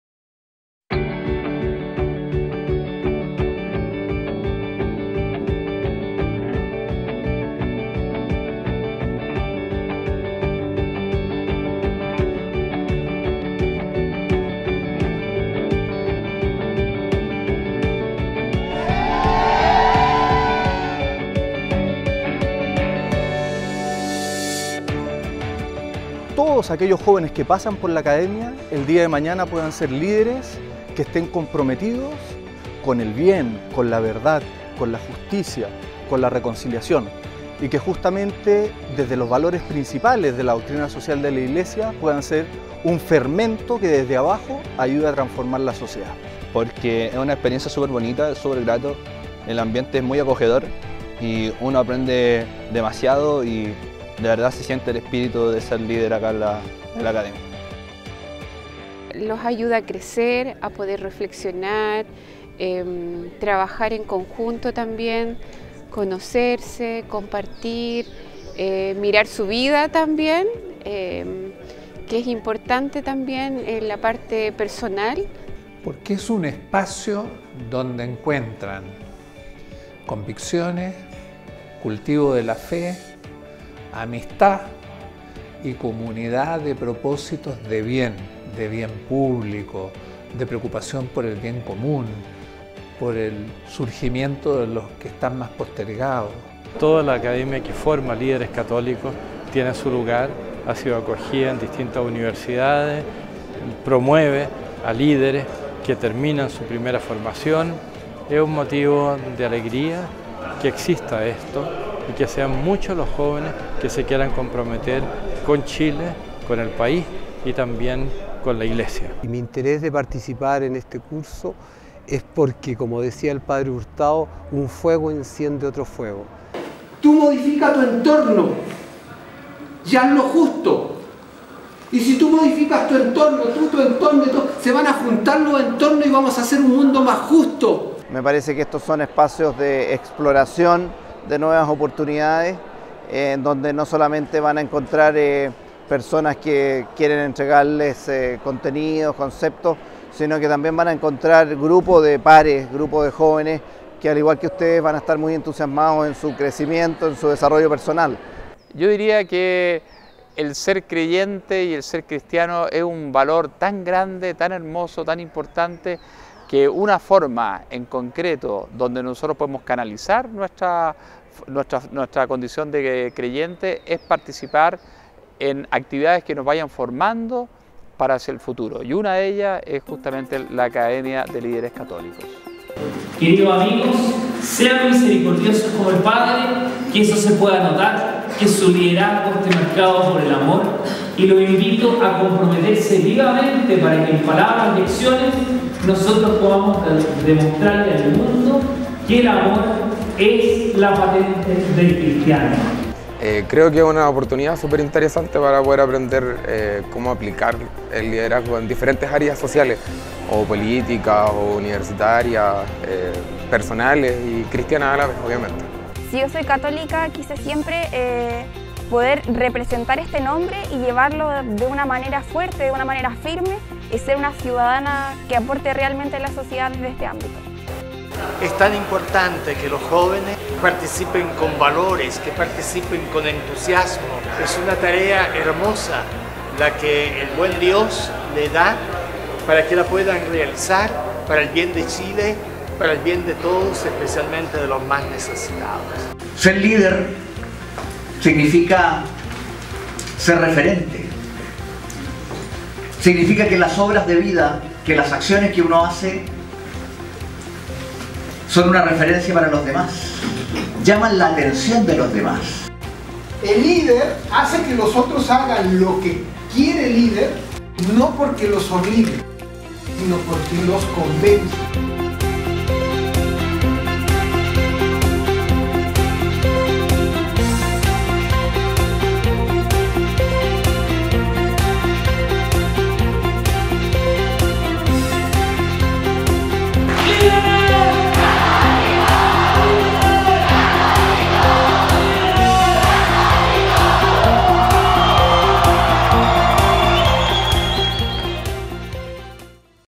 Líderes Católicos 2017
Video Institucional 2017
Formando Líderes Católicos al más alto nivel con más de 3000 egresados